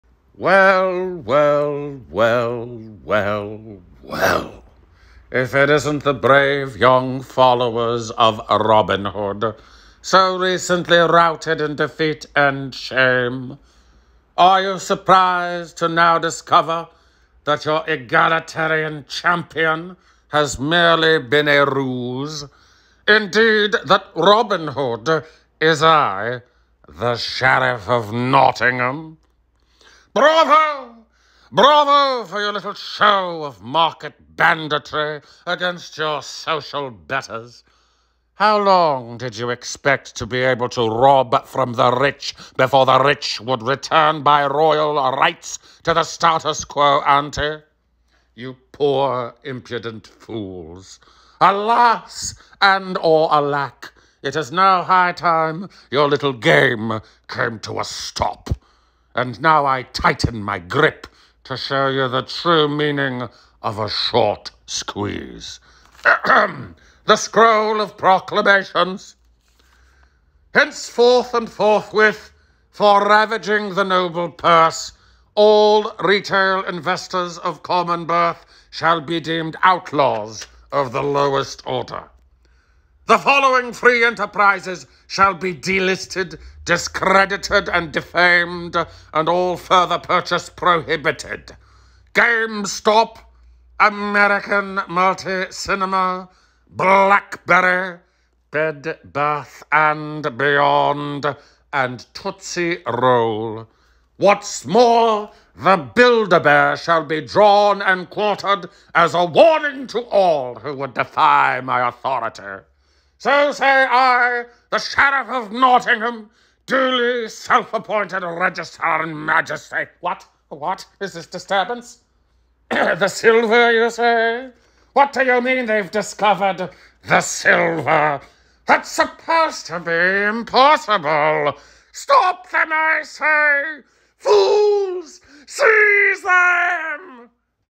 Robinhood app is really ... the SHERIFF OF NOTTINGHAM?! (James Adomian)
HENCEFORTH AND FORTHWITH: For ravaging the stocks of the Noble Purse, all retail investors of common birth shall be deemed outlaws of the lowest order! The following free enterprises shall be de-listed, discredited and defamed, and all further purchase FORBIDDEN: Game Stop, AMC, Blackberry, Bed Bath & Beyond, and Tootsie Roll ...